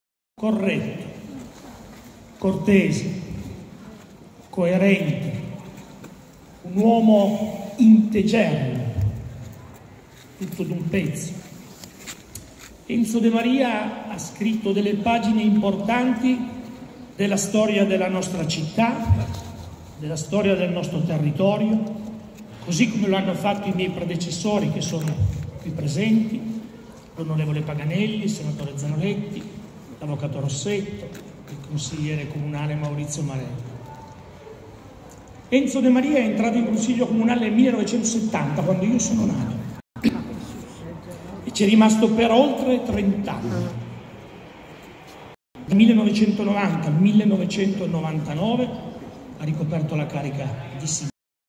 Lutto cittadino ad Alba - Carlo Bo
LUTTO CITTADINO ⚪  Grande cordoglio alla cerimonia funebre di Enzo Demaria, nel primo pomeriggio di oggi, 1 marzo. Il  funerale, celebrato in Duomo, è stato precededuto da un momento di ricordo sotto i portici del palazzo comunale, con le bandiere a mezz’asta in segno di lutto cittadino 🎤 Ecco il ricordo dell'attuale sindaco Carlo Bo nel video di Francesca Pinaffo